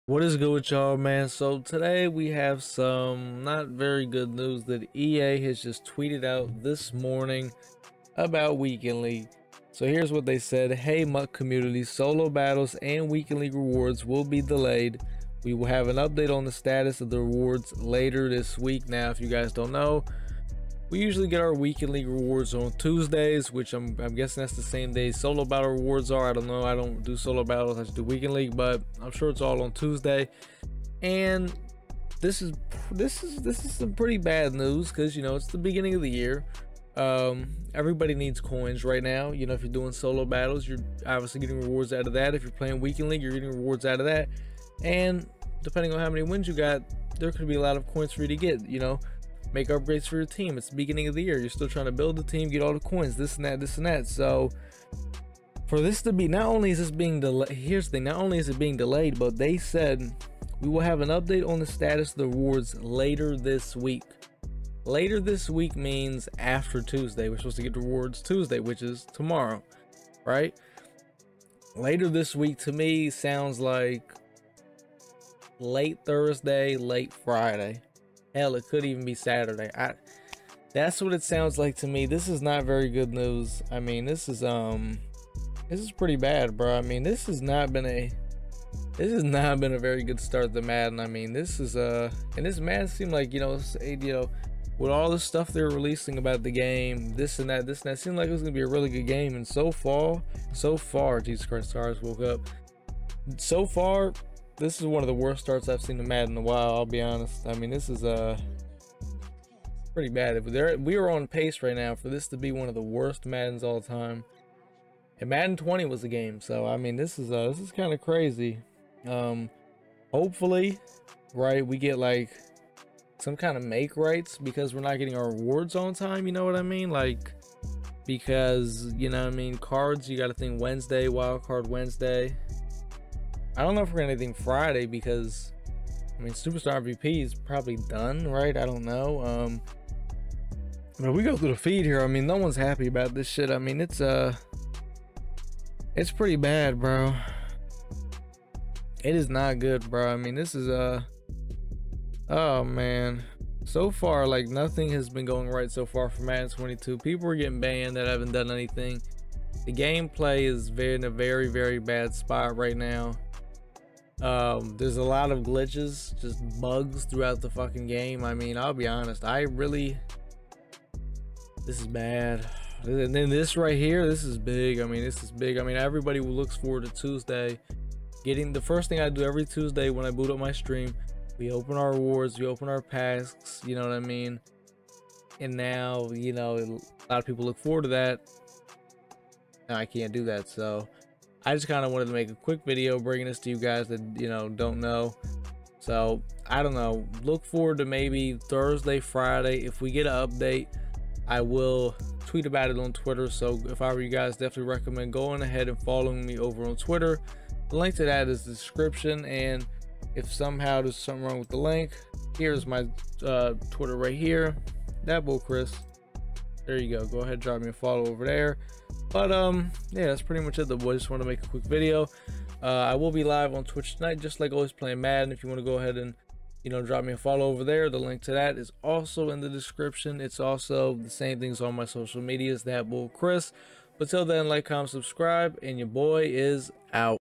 WEEKEND LEAGUE REWARDS ARE GETTING PUSHED BACK TO THE WEEKEND
Follow the links down below to stay up to date with videos